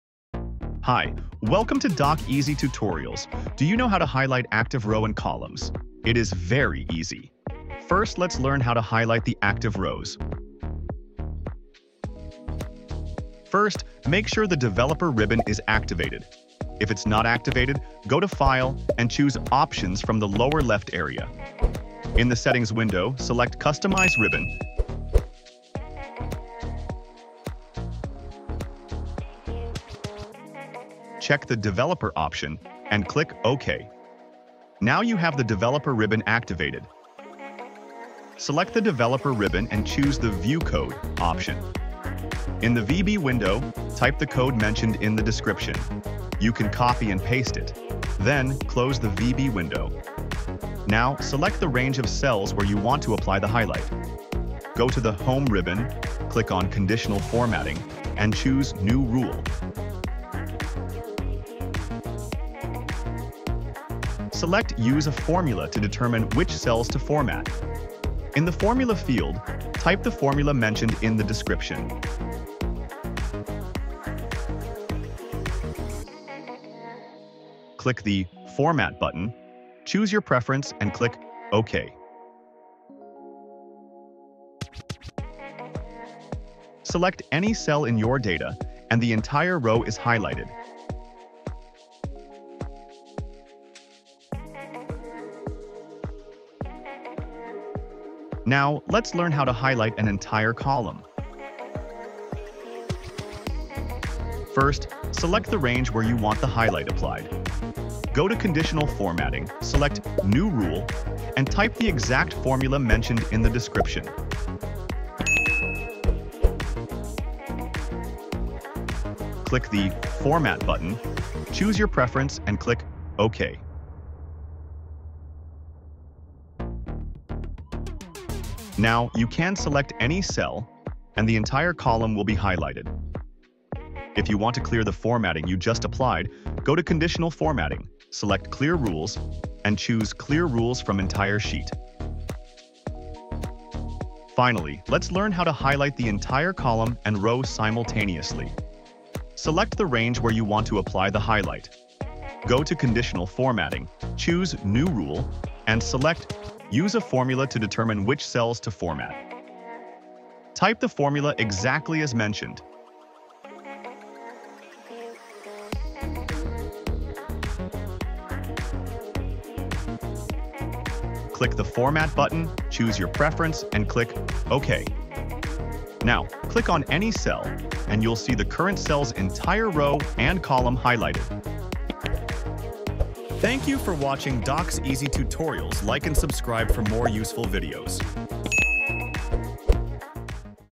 How to Highlight Active Row and Column in Excel: Step-by-Step Tutorial
Learn How to Highlight Active Cells, Rows, and Columns in Excel!

In this step-by-step tutorial, we'll show you how to highlight the active cell, entire row, and entire column in Excel. This technique is perfect for improving your data analysis and making your spreadsheets more readable.

What you'll learn:

Activating the Developer Ribbon in Excel.
Using VBA code to highlight the active cell.
Applying Conditional Formatting to highlight entire rows and columns.

Steps covered in the video:

Activating the Developer Ribbon:
Go to File, then Options, then Customize Ribbon, check "Developer", and click "OK".
Adding VBA Code:
Open the Developer Ribbon, click "View Code", enter the provided VBA code, and close the VB window.
Using Conditional Formatting:
Select the cell range, go to Home Ribbon, Conditional Formatting, New Rule, Use a formula to determine which cells to format, and enter the provided formula.
Formulas and Codes Mentioned:

VBA Code:
(Private Sub Worksheet_SelectionChange(ByVal Target As Range)
If Application.CutCopyMode = False Then
Application.Calculate
End If
End Sub)

Conditional Formatting Formula:
1. Highlight Entire Row: =CELL("ROW")=ROW()
2. Highlight Entire Column: =CELL("COL")=COLUMN()
3. Highlight Both Row and Column simultaneously: =OR(CELL("ROW")=ROW(),CELL("COL")=COLUMN())